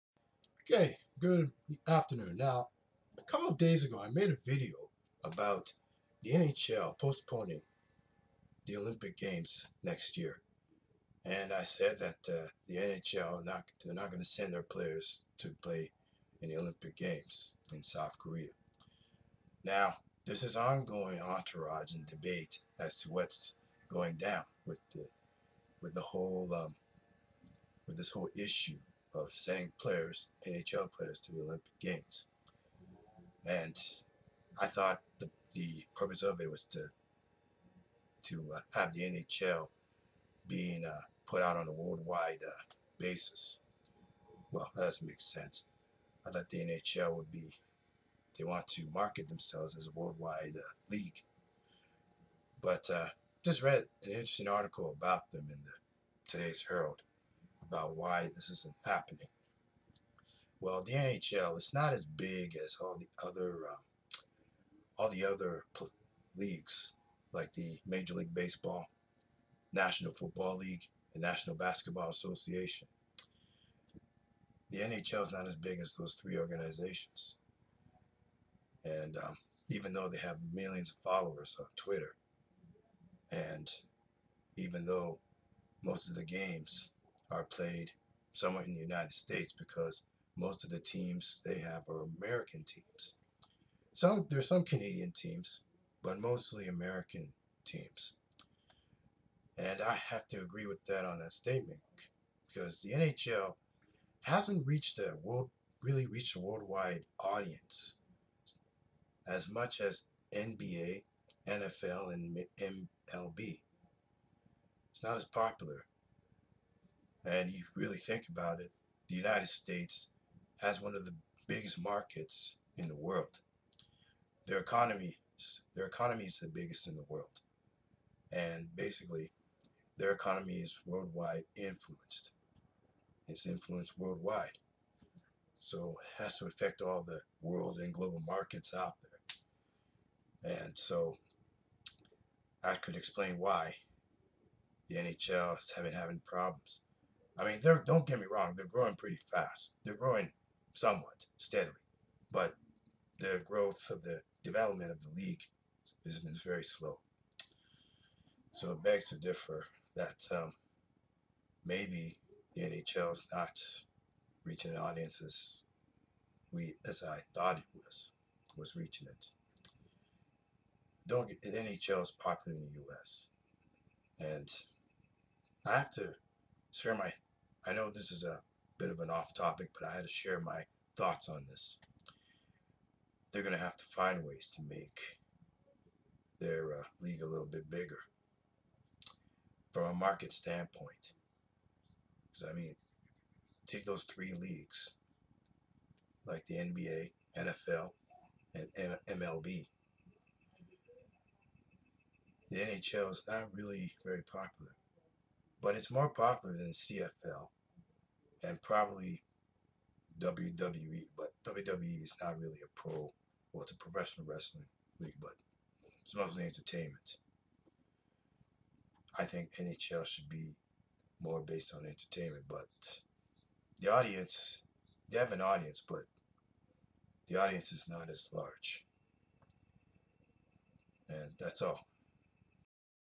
NHL And Global Markets
The league has a long ways to go before being able to reach a large global market like other pro leagues.